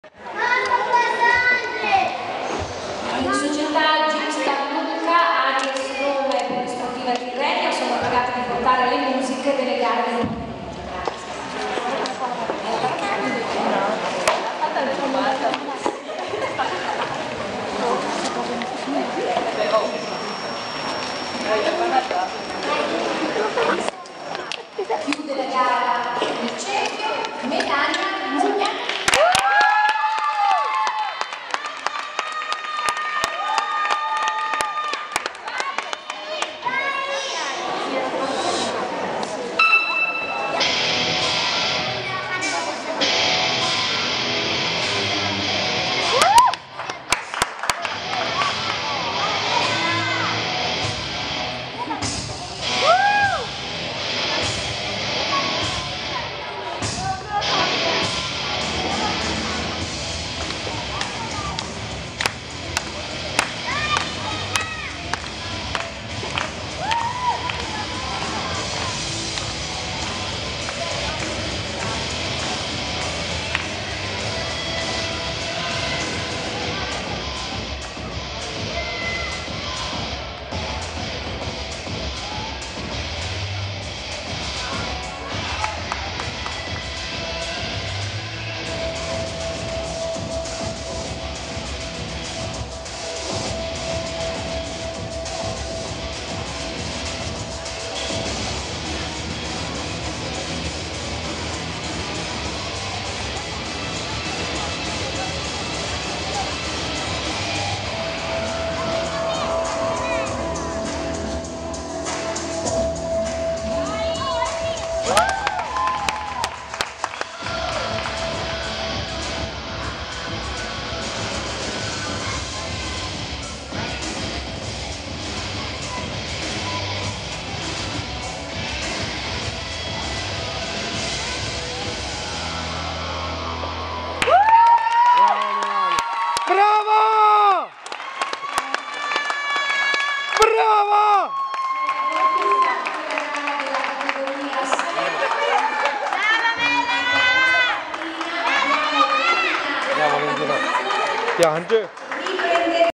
Melania Mugnai - Cerchio - Campionati Nazionali AICS 2013
Melania Mugnai, della Polisportiva Tirrenia, si è classificata 2° con l'attrezzo cerchio ai Campionati Nazionali svolti a Lignano Sabbiadoro dal 24 al 26 maggio 2013.